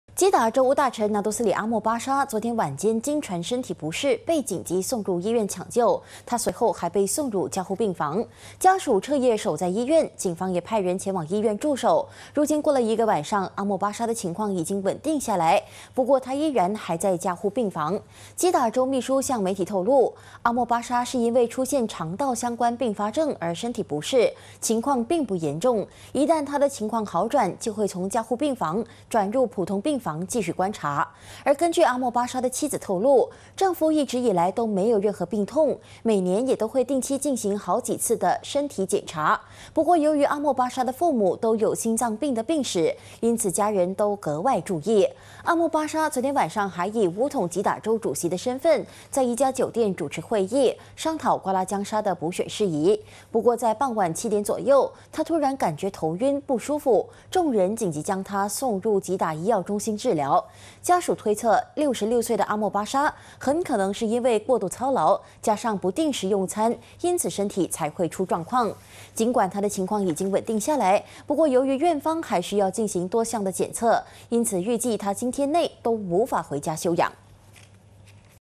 吉大臣昨紧急入加护病房 今情况稳定 [20160605]
吉打州务大臣拿督斯里阿末巴沙昨天晚间惊传身体不适，被紧急送入医院抢救，他随后还被送入加护病房。家属彻夜守在医院，警方也派人前往医院驻守。如今过了一个晚上，阿末巴沙的情况已经稳定下来，不过他依然还在加护病房。